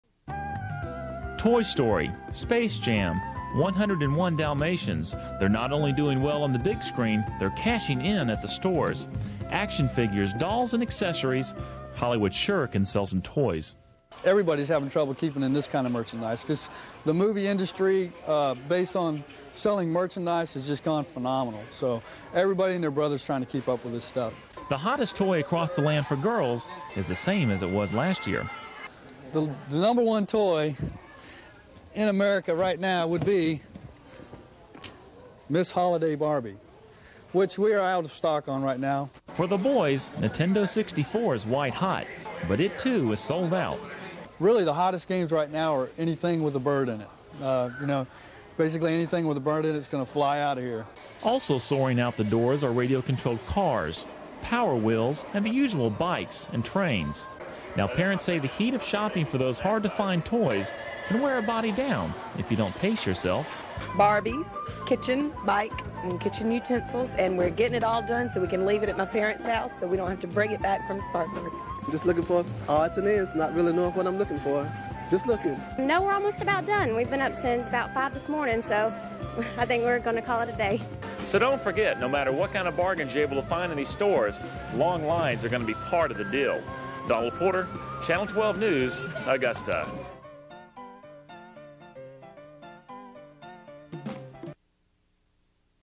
Hottest Toys for Christmas '96 | WRDW-12 Augusta (11/21/1996)
Barbies, Nintendo 64, and anything with a bird in it!